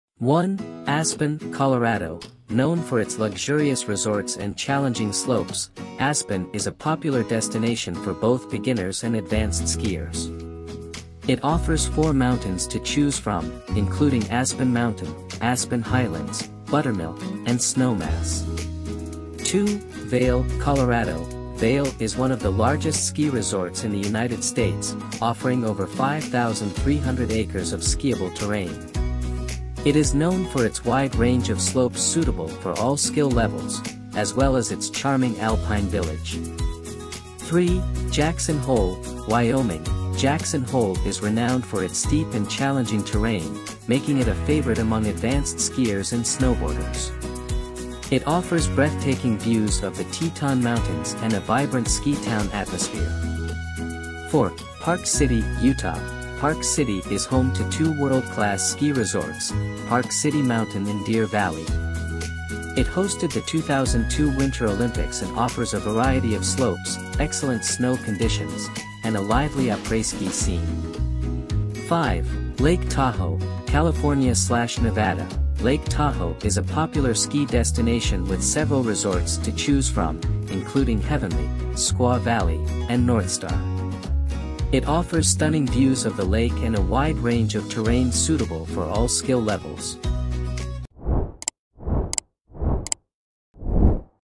Ski Like a Pro: The 5 Best Ski Resorts in the USA for 2024
1. Aspen, Colorado: Known for its luxurious resorts and challenging slopes, Aspen is a popular destination for both beginners and advanced skiers. It offers four mountains to choose from, including Aspen Mountain, Aspen Highlands, Buttermilk, and Snowmass.

2. Vail, Colorado: Vail is one of the largest ski resorts in the United States, offering over 5,300 acres of skiable terrain. It is known for its wide range of slopes suitable for all skill levels, as well as its charming alpine village.

3. Jackson Hole, Wyoming: Jackson Hole is renowned for its steep and challenging terrain, making it a favorite among advanced skiers and snowboarders. It offers breathtaking views of the Teton Mountains and a vibrant ski town atmosphere.

4. Park City, Utah: Park City is home to two world-class ski resorts, Park City Mountain and Deer Valley. It hosted the 2002 Winter Olympics and offers a variety of slopes, excellent snow conditions, and a lively après-ski scene.

5. Lake Tahoe, California/Nevada: Lake Tahoe is a popular ski destination with several resorts to choose from, including Heavenly, Squaw Valley, and Northstar. It offers stunning views of the lake and a wide range of terrain suitable for all skill levels.best all mountain skis